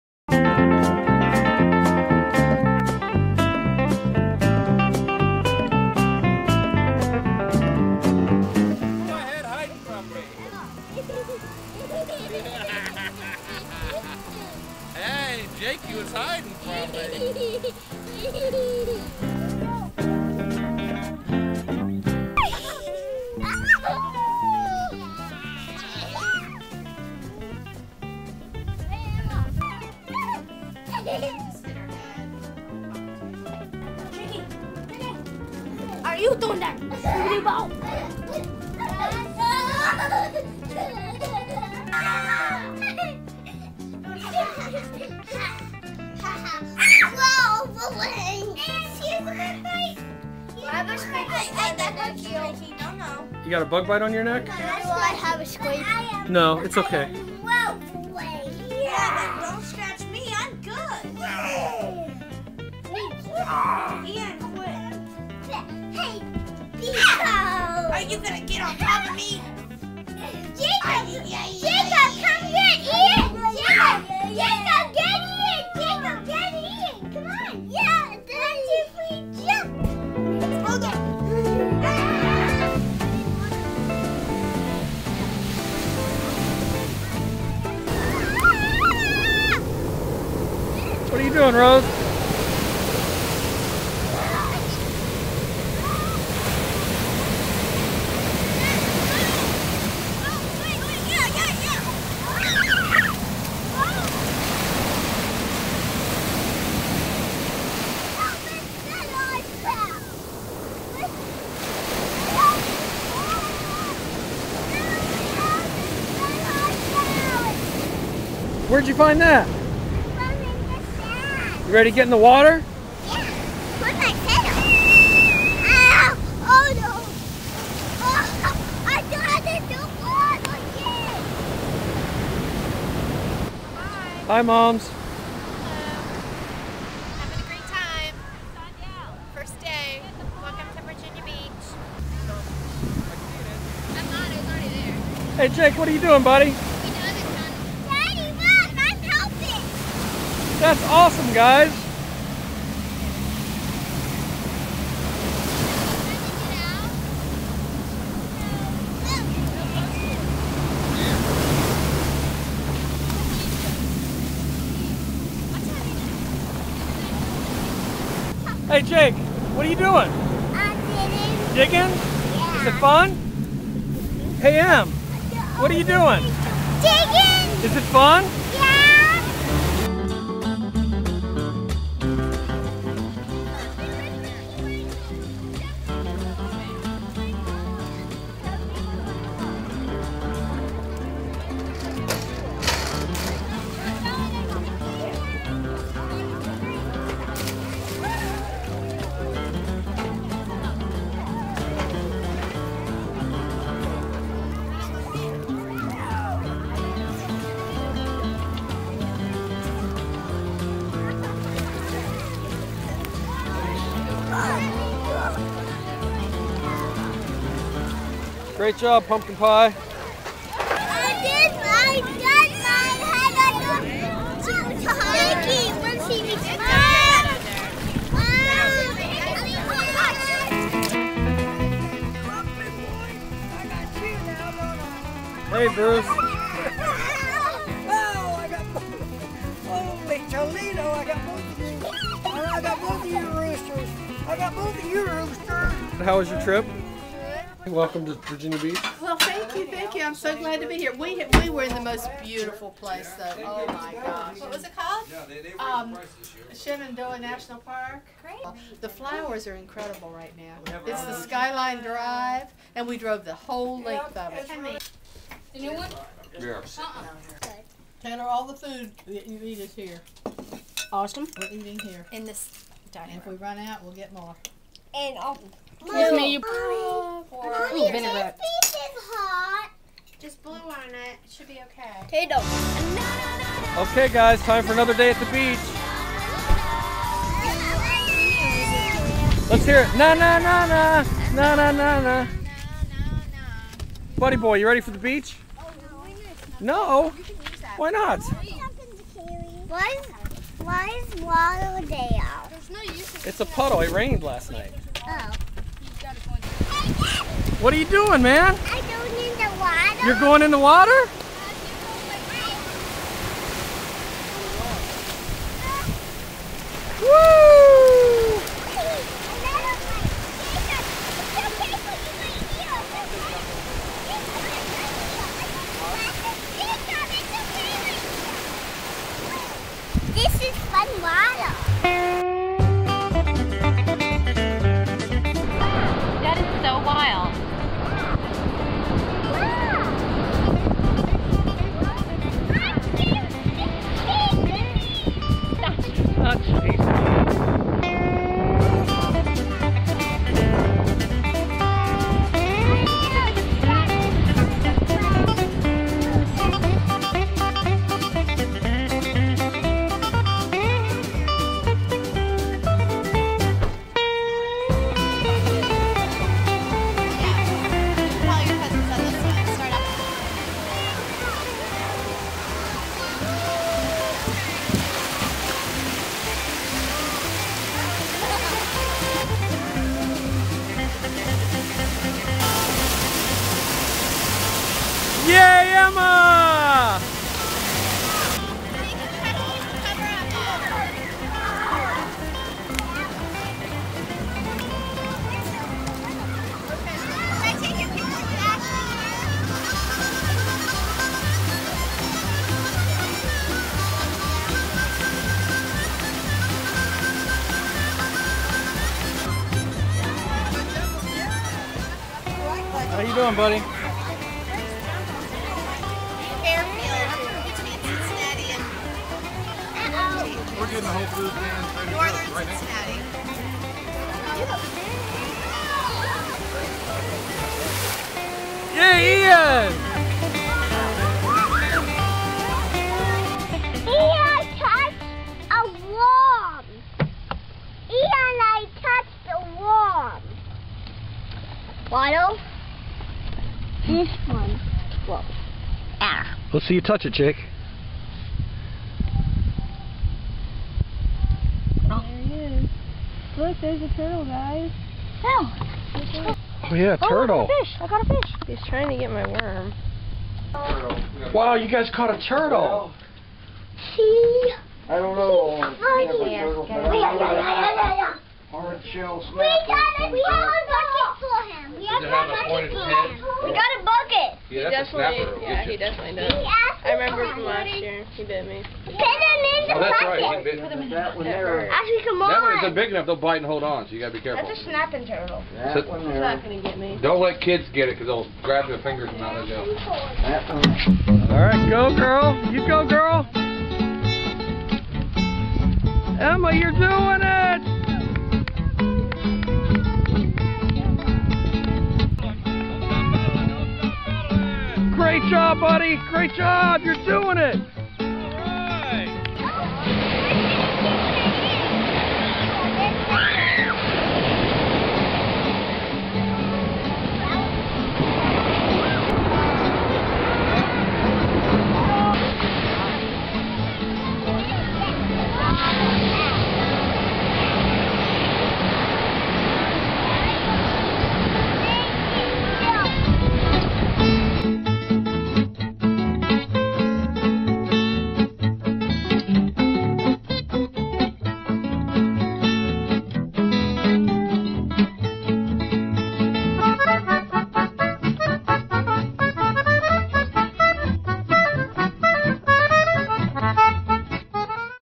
2008 Donnellan Virginia Beach Vacation
For the second year, the Donnellan family comes to Virginia Beach for summer fun!